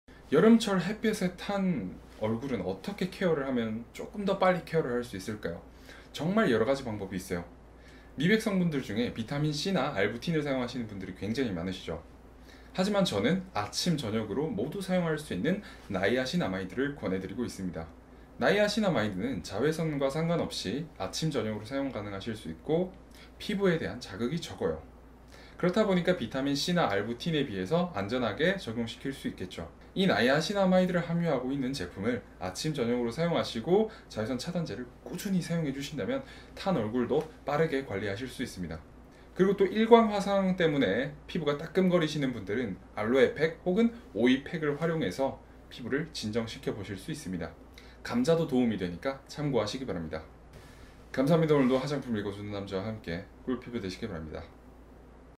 [1분팁] 탄 얼굴 관리하는 팁
자외선에 탄 얼굴을 관리하는 팁을 공개합니다.

전국민 꿀피부 프로젝트에 동참해주세요!